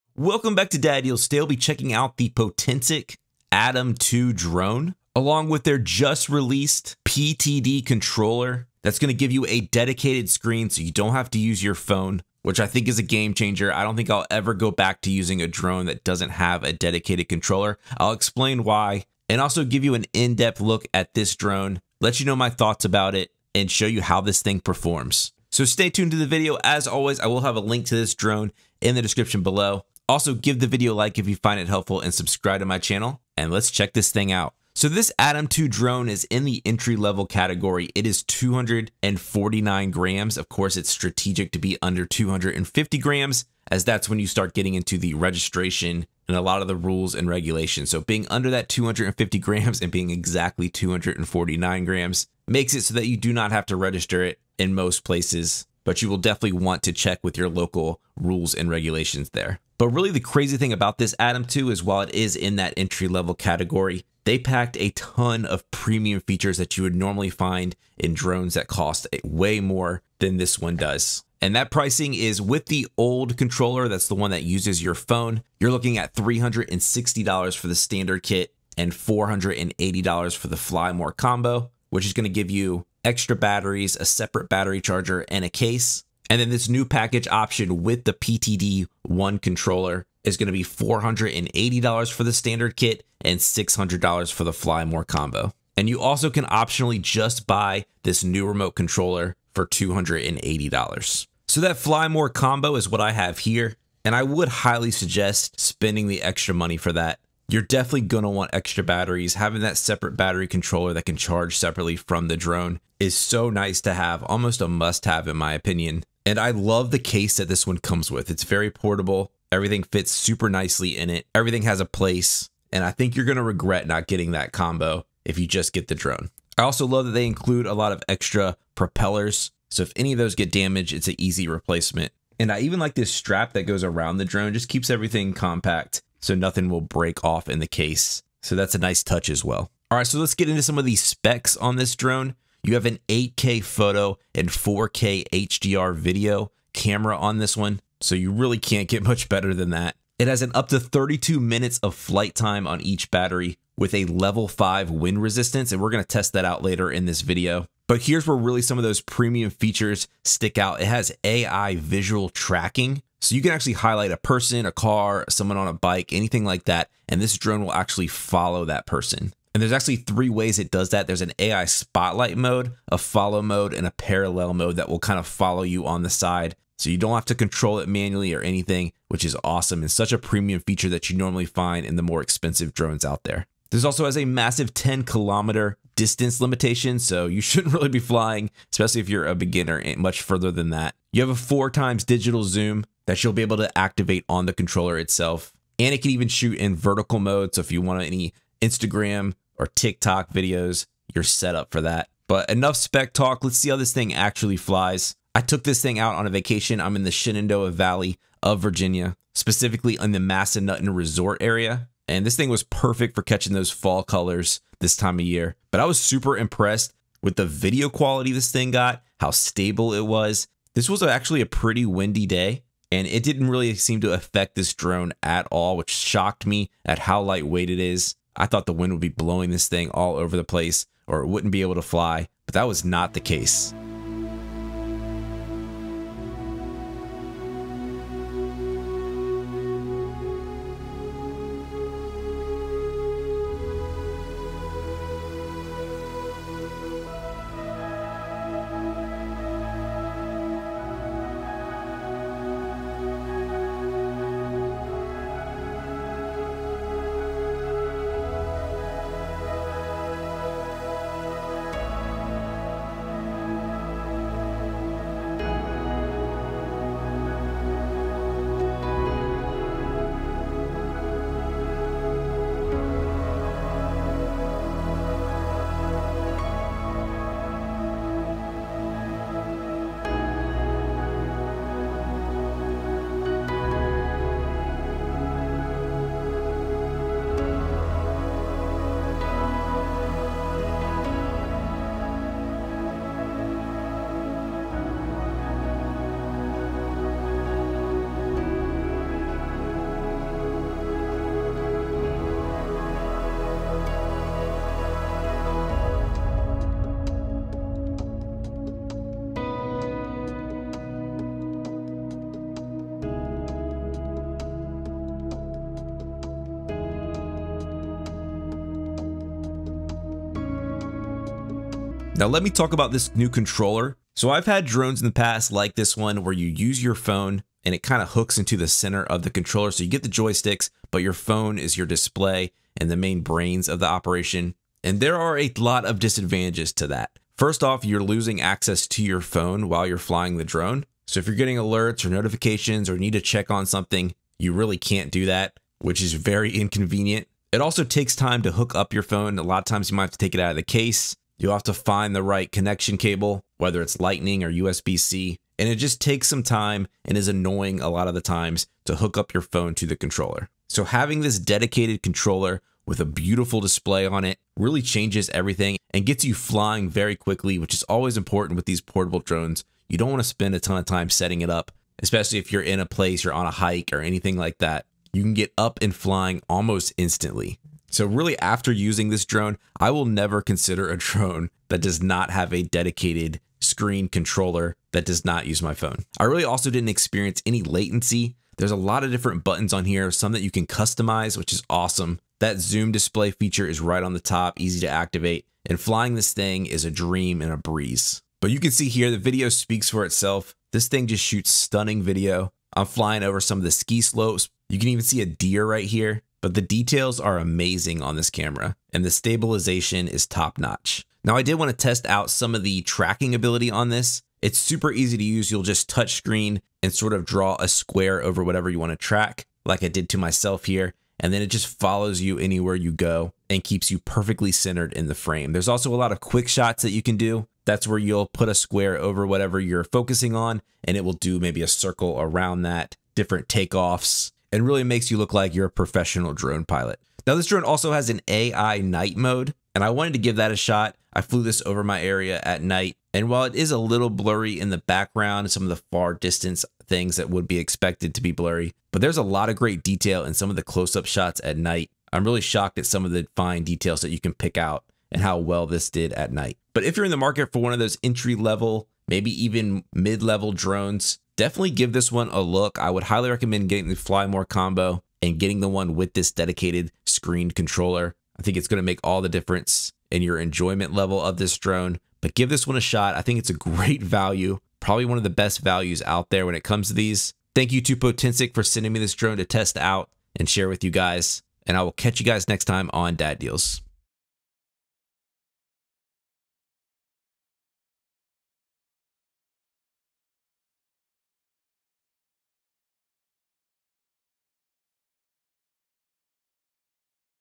Potensic Atom 2 Drone & Upgraded Controller Review!
In today's video, we're taking a deep dive into the Potensic Atom 2 drone – but with a twist! I'm also reviewing their brand new controller to see how it enhances the flying experience.

Is this budget-friendly drone still a top contender, especially with the upgraded controller? We'll cover everything from unboxing and setup to flight performance, camera quality, and battery life. I'll share my honest thoughts on whether this combo is worth your hard-earned cash.

Whether you're a seasoned drone pilot or looking for your first drone, you won't want to miss this comprehensive review.